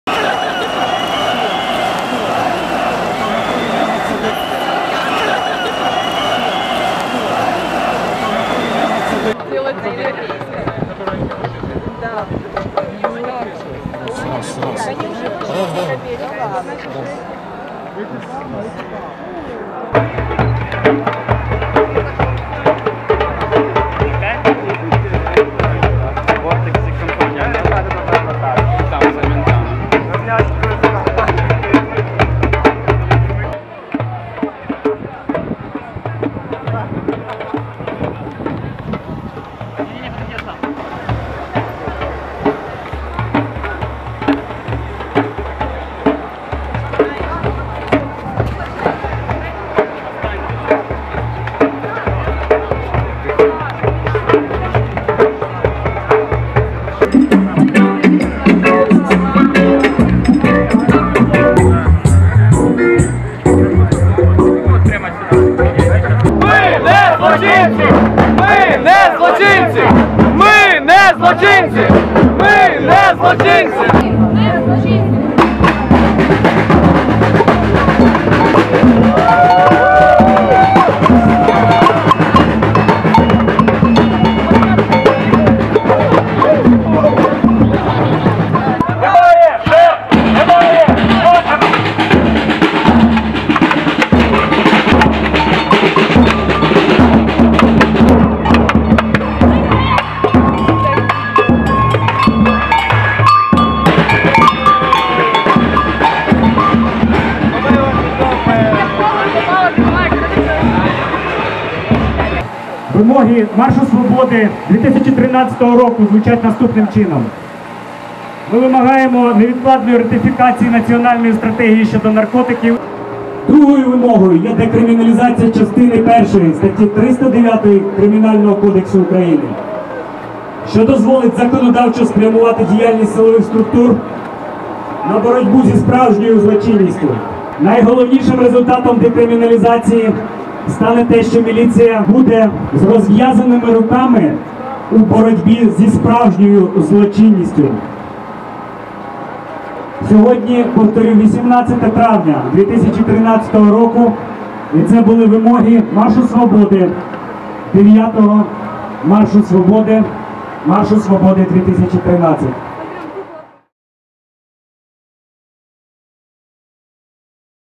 Freedom March 2013 in Kiev
Annual Freedom March in Kiev in Ukraine for liberalization of drug laws and decriminalization of drug possesion.